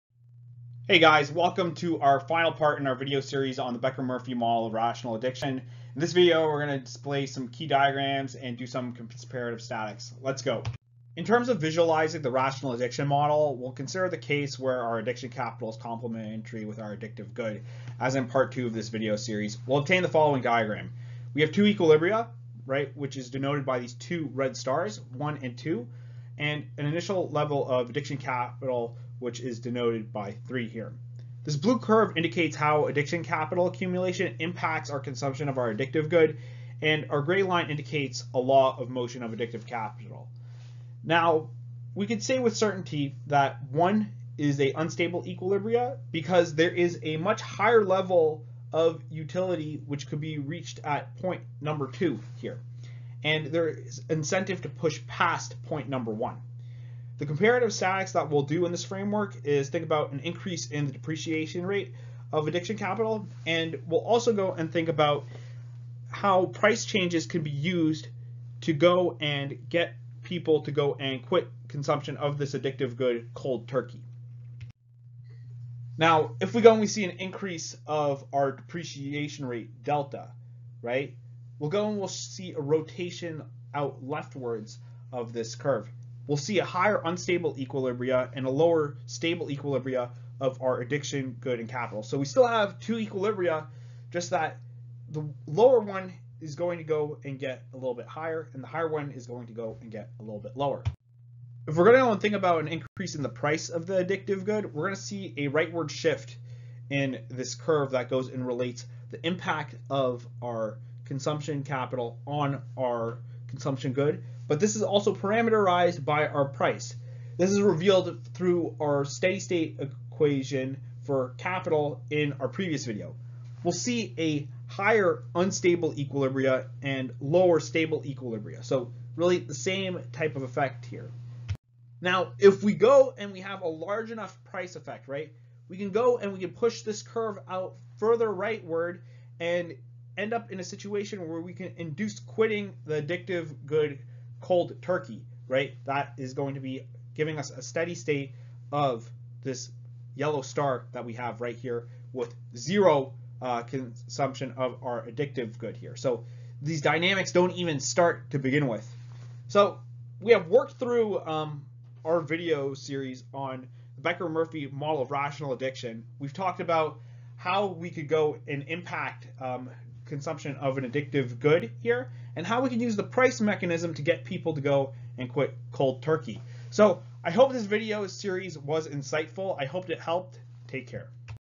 The Becker Murphy Model of Rational Addiction: Part 3/3
In this video we present the phase diagram relevant for the rational addiction model and do some comparative statics with it.